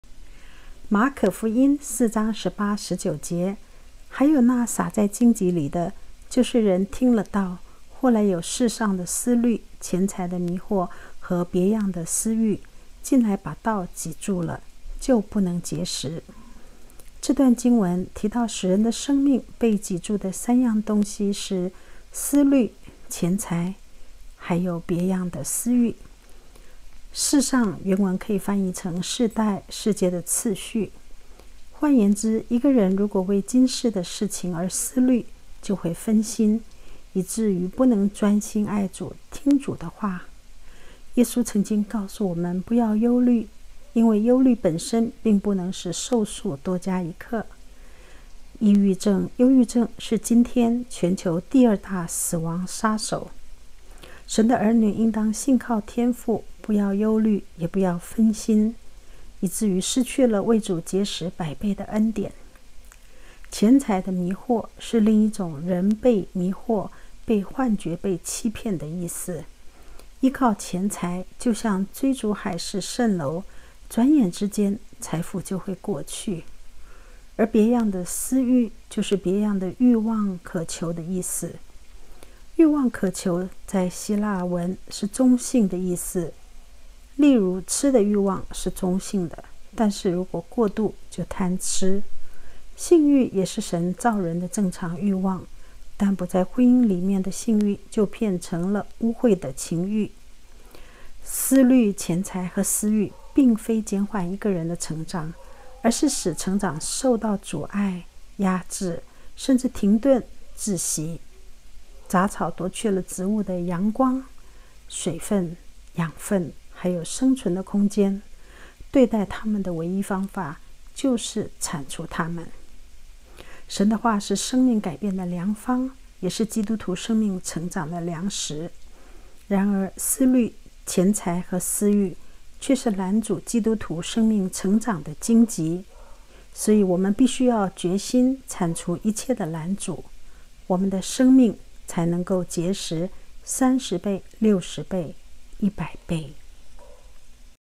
馬可福音14
‪馬可福音4:18,19‬
‪還有那撒在荊棘裡的、就是人聽了道．後來有世上的思慮、錢財的迷惑、和別樣的私慾、進來把道擠住了、就不能結實。‬

這段經文，提到使人的生命被擠住的三樣東西是：思慮（anxiety），錢財（affluence）與別樣的私慾。

●世上的思慮：「世上」原文是αἰών，可譯成「世代、世界的秩序、永遠」。換言之，一個人如果為今世的事而思慮，就會分心，以至於不能專心愛主﹑聽主的話！

耶穌曾經告訴我們「不要憂慮」，因為，憂慮本身並不能使壽數多加一刻呢？（馬太福音六25-27）郁鬱症（憂鬱症）是今天全球第二大死亡殺手，神的兒女應當信靠天父，不要憂慮，也不要分心，失去了為主結實百倍的恩典！

●錢財的迷惑：另一種人是被錢財迷惑了，「迷惑」在希臘文是「幻覺﹑欺騙」的意思。倚靠錢財就像追逐海市蜃樓，轉眼之間，財富就會過去。

●別樣的私慾：希臘文是ἐπιθυμία，有「慾望，渴求」之意，在希臘文一般是中性的意義。例如「吃慾」是中性的，但如果過度就是「貪吃」，又如「性慾」，原本是神造人的正常慾望，但不在婚姻裡面的「性慾」，就成為了「污穢的情慾」(彼後2:10)、思慮﹑錢財和私慾並非減緩一個人的成長；而是使成長受到阻滯或壓抑，以至停頓與窒息。雜草奪去了植物的陽光﹑水﹑養分和生存空間。對待它們的唯一方法，就是鏟除它們。

神的話是生命改變的良方，也是基督徒生命成長的糧食。然而，「思慮」﹑「錢財」和「私慾」卻是攔阻基督徒生命成長的荊棘，所以我們必須要決心剷除一切的攔阻，我們的生命才能結實30，60，100倍！